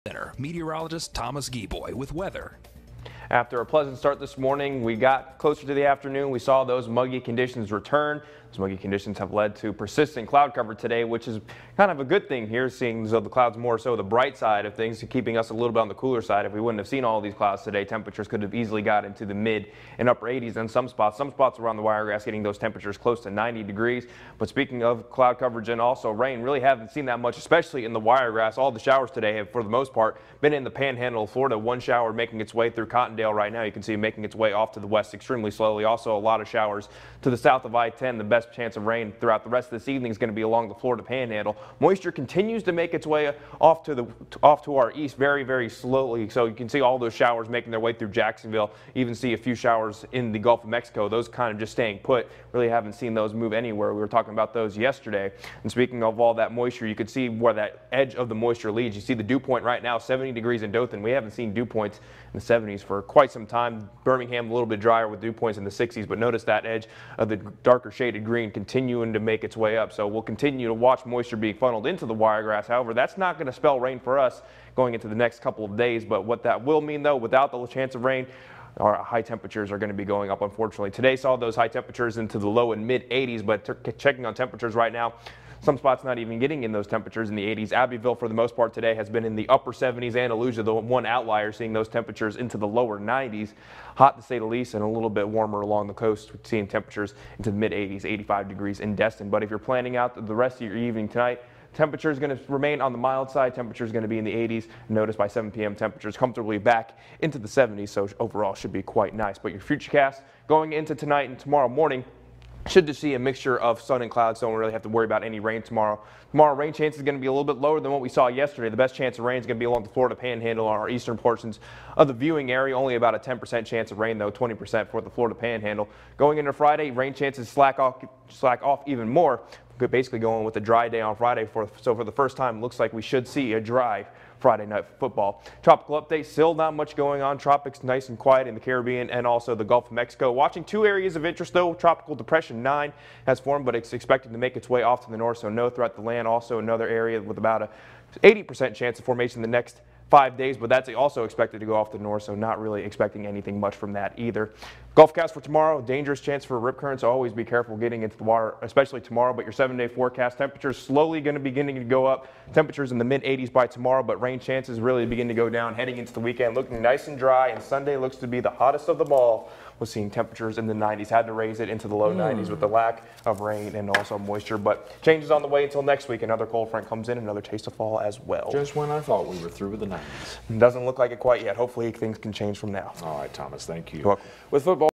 Wednesday Afternoon Weather
WRGX First News at 4 Main Weather:
The muggy conditions have returned, they look to be staying into the weekend.
Plus trouble brewing in the tropics.. A new depression has formed.